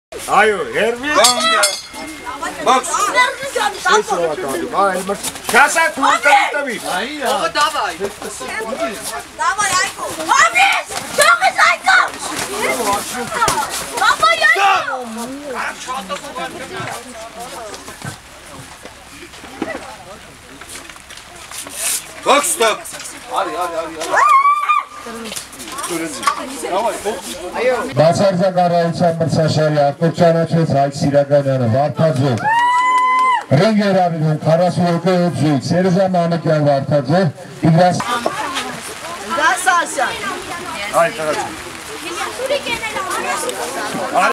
50կգ Հովհաննես Հակոբյան (Չեմպիոն ԲԱ)-Հայկ Սիրականյան (Մարտունի)
Բռնցքամարտի առաջնություն նվիրված Գեղարքունիքի մարզում բռնցքամարտի ֆեդերացիայի կազմավորման 9-րդ տարեդարձին 50կգ Հովհաննես Հակոբյան (Չեմպիոն ԲԱ)-Հայկ Սիրականյան (Մարտունի) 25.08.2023թ. Շողակաթ